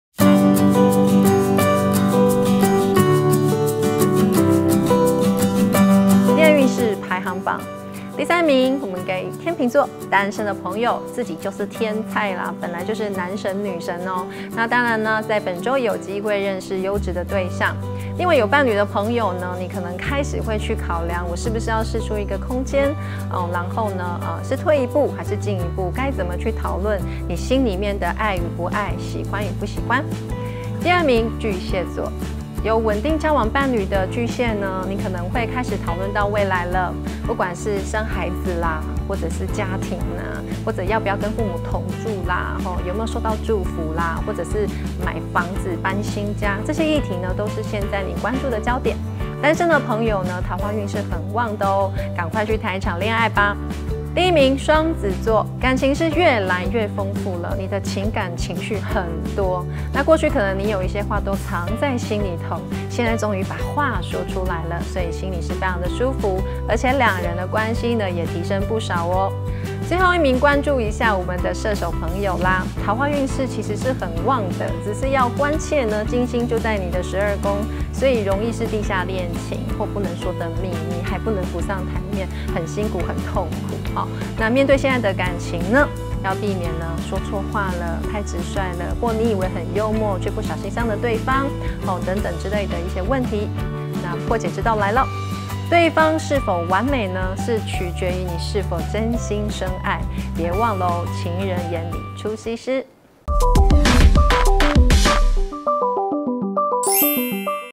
天秤易遇到天菜？！ 射手當心說話傷人｜17VIDEO｜Useful Astrology Tips You Should Know for the Upcoming Week
本週戀愛運勢排行榜第三名的天秤寶寶有機會遇在天菜條件的優質對象；有另一半的天秤座與伴侶相處上的問題，會勇於為自己發聲。

第二名的巨蟹座與穩定交往的伴侶將討論到未來家庭、房子或雙方父母的議題；單身朋友面露桃花，不破單好可惜唷！

恭喜第一名的雙子座感情世界越發豐富，單身者有利職場桃花的發展。

最後一名的是射手座，射手座的朋友面對現在的戀情，要避免直率而說錯話，不小心傷了對方。